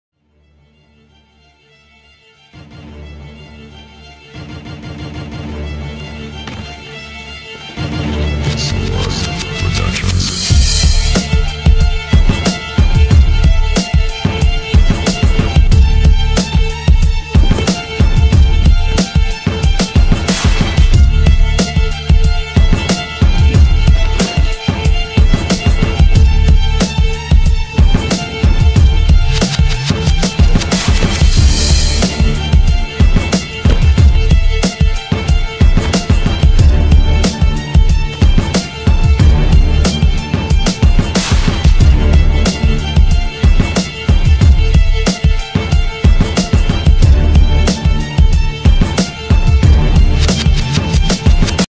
Snowscoot in Toxopark
Небольшой замес в @toxopark .Этот год начался с отличной зимы и я стараюсь не упускать возможности насладиться ей! Радуйся каждой минуте!